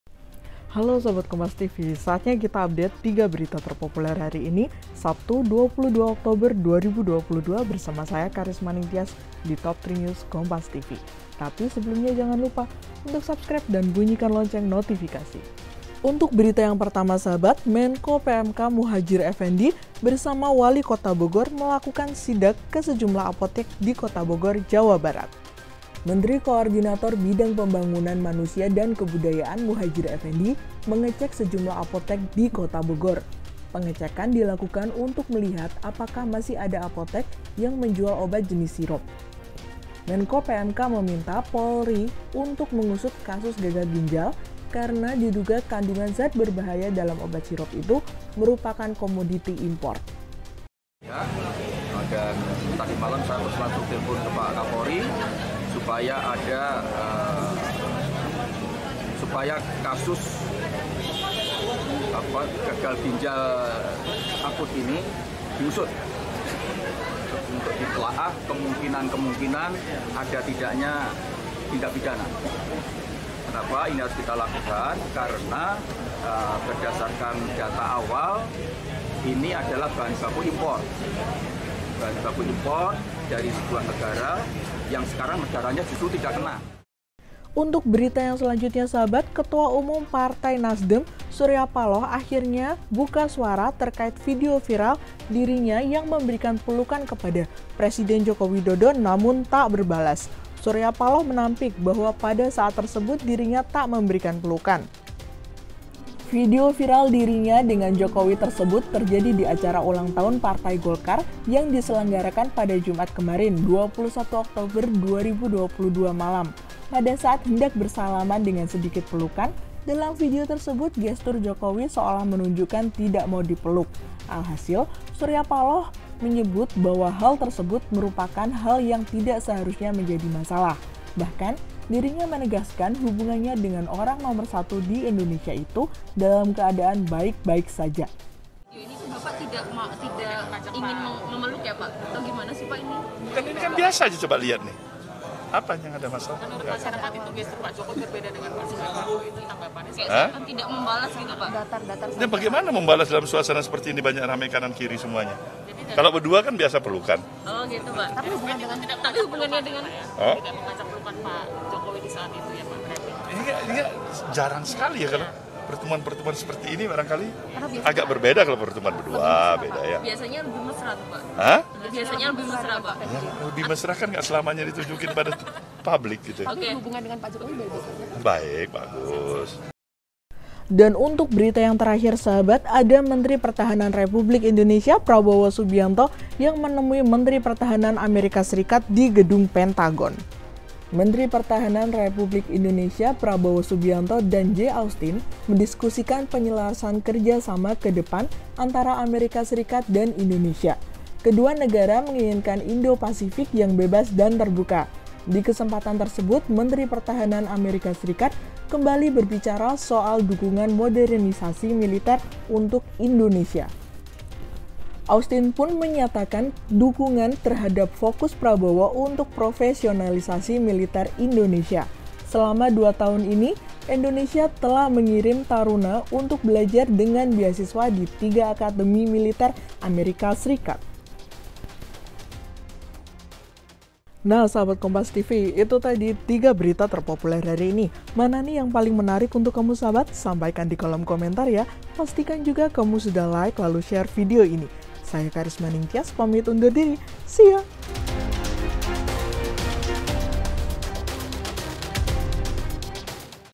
[TOP 3 NEWS] Menko PMK Sidak Apotek, Viral Jokowi Enggan Peluk Surya Paloh, Prabowo Ketemu Menhan AS
JAKARTA, KOMPAS.TV  Berita pertama, Menteri Koordintor Bidang Pembangunan Manusia dan Kebudayaan Muhadjir Effendy mengecek sejumlah apotek di kota Bogor. Pengecekan dilakukan untuk melihat apakah masih ada apotek yang menjual obat jenis sirop. Menko PMK meminta Polri mengusut kasus gagal ginjal karena diduga kandungan zat berbahaya dalam obat sirop itu merupakan impor.

Baca Juga Kabar Baik! Solusi Gagal Ginjal Akut, Pemerintah Datangkan Obat "Fomepizole" dari Singapura di

Berita kedua, Beredar viral video potongan pertemuan Surya Paloh dengan Presiden Jokowi di HUT Partai Golkar di Twitter. Video tersebut menarasikan gestur Jokowi yang seolah enggan dipeluk Surya Paloh. Menurut Surya Paloh tak ada hal yang perlu dipermasalahkan. Lebih lanjut Surya Paloh mengatakan kemesraan tidak selalu harus ditunjukkan ke publik.

Baca Juga Saat Jokowi Kaget Lihat Luhut Pakai Jas Kuning di HUT Golkar: Biasanya Pakai Batik di

Berita ketiga, Menteri Pertahanan RI Prabowo Subianto temui Menteri Pertahanan AS Lloyd J. Austin di Gedung Pentagon, Kamis (20/10) waktu setempat. Prabowo dan Austin mendiskusikan penyelarasan kerja sama ke depan antara AS dan Indonesia di mana kedua negara menginginkan Indo-Pasifik yang bebas dan terbuka.